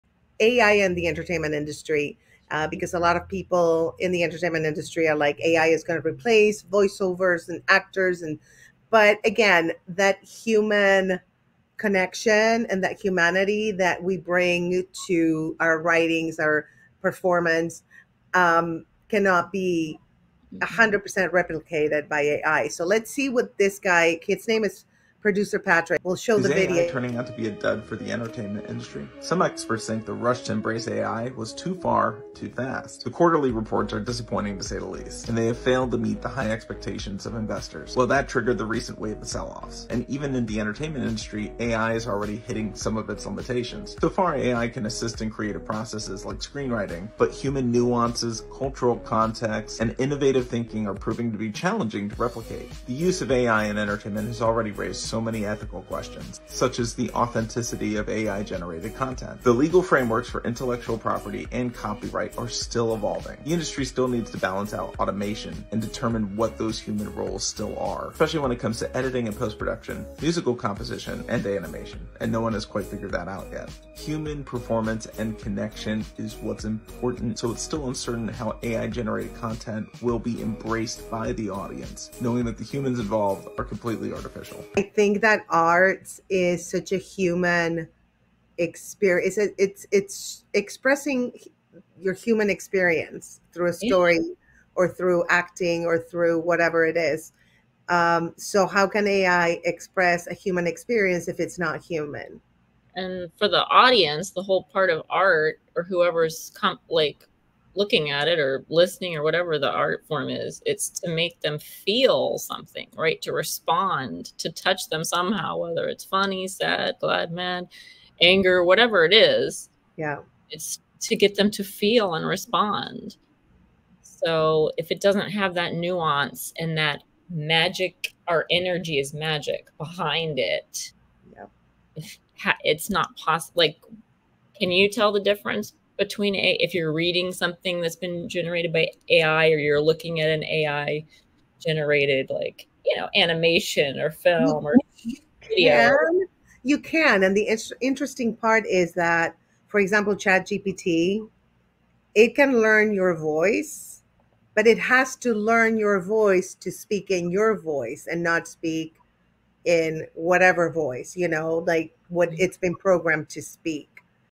🎨🤖 Unleashing Creativity: AI & Art on Brain Babes Podcast! 🎙✨
Get ready to dive into the fascinating world where technology meets creativity! In this episode of Brain Babes, we're exploring the incredible fusion of Artificial Intelligence and Art. 🌟✨

Discover how AI is transforming the art scene, pushing boundaries, and inspiring new forms of creative expression.

Learn about the latest trends, groundbreaking projects, and the future of art in the digital age. Whether you're an art enthusiast, tech geek, or just curious about the magic of AI, this episode is packed with insights, inspiration, and a whole lot of fun! 🎨🤩